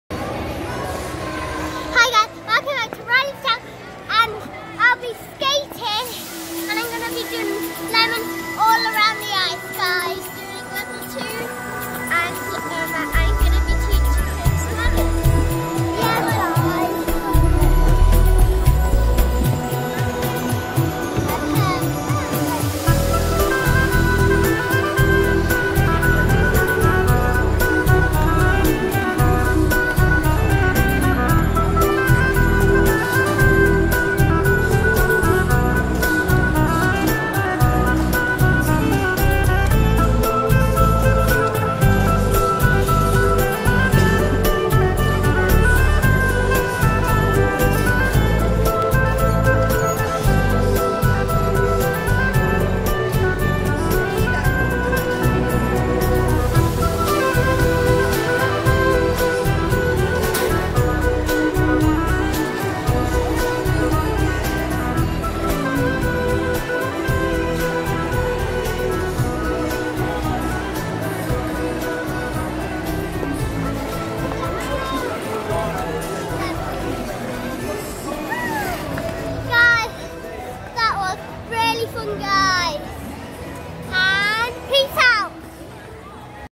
Riley On Ice @ Ice Sheffield with Big Sis Danielle On Ice
I just passed my Level 2 Skate UK at Ice Sheffield in yes you guessed it SHEFFIELD, ive got my Lemons (twizzles) now Guys.